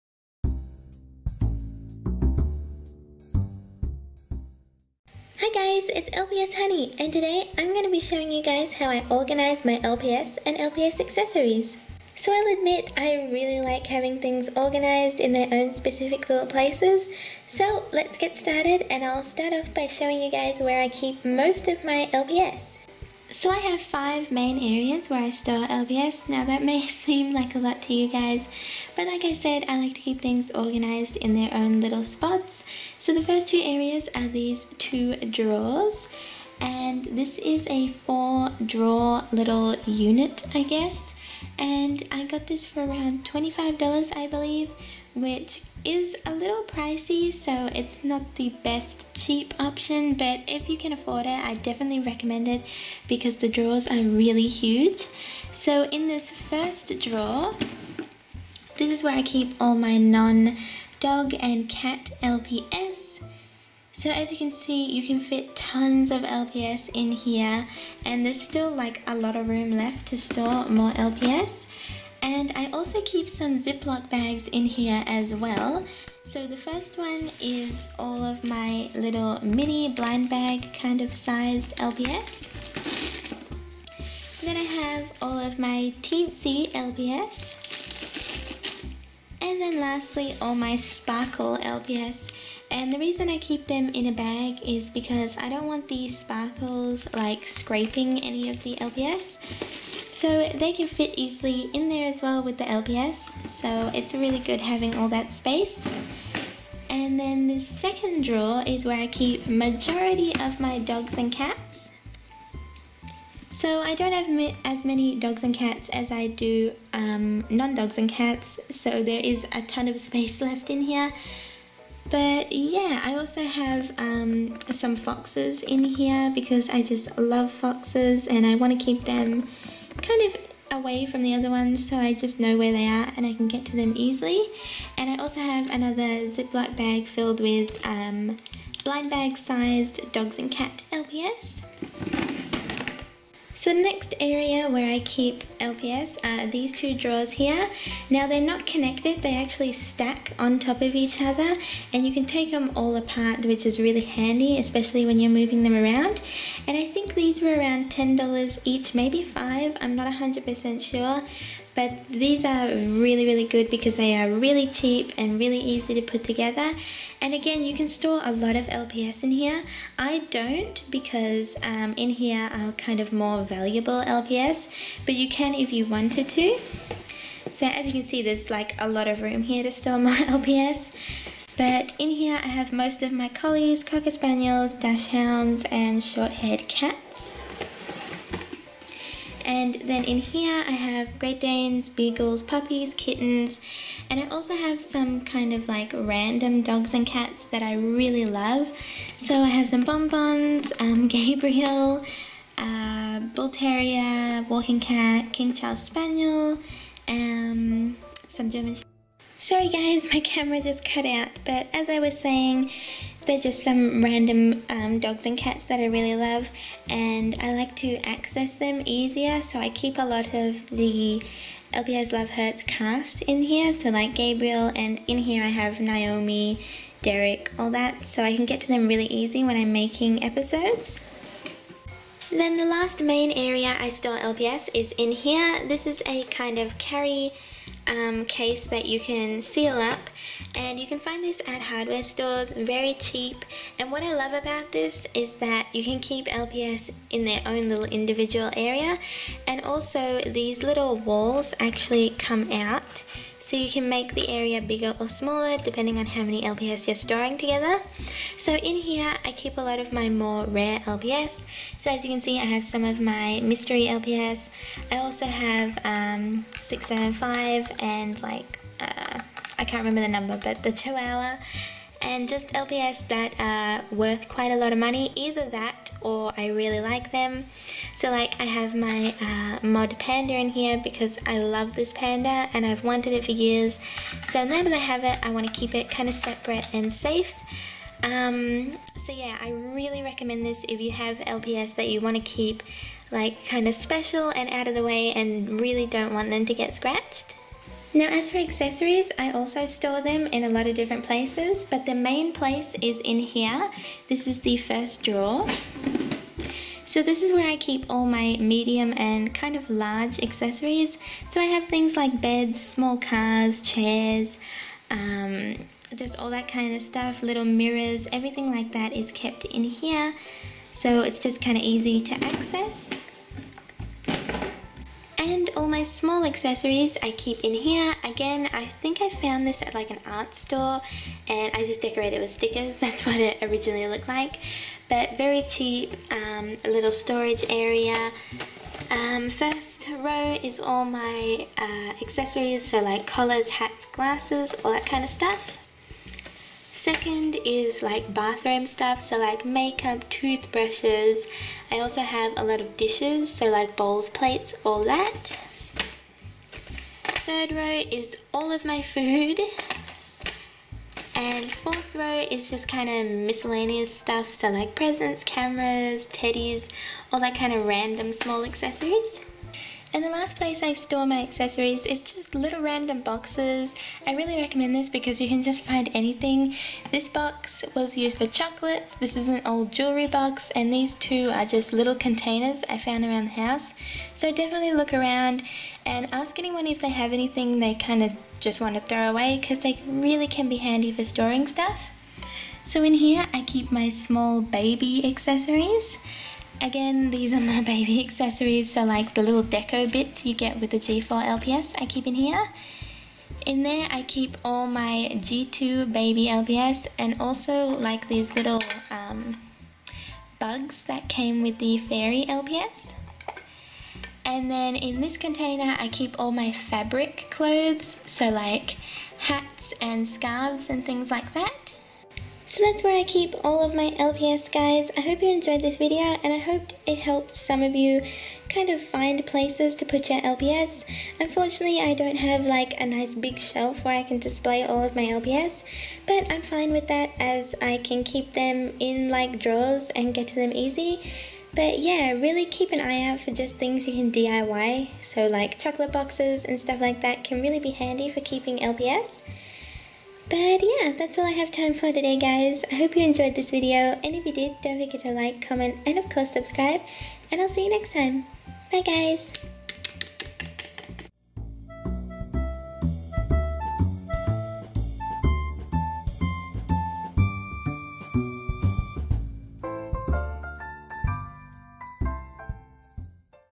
How I Organise My Lps & Accessories || DIY (Kinda)
The time has come for me to show you guys where I keep all my lps and lps accessories, and also a few tips :D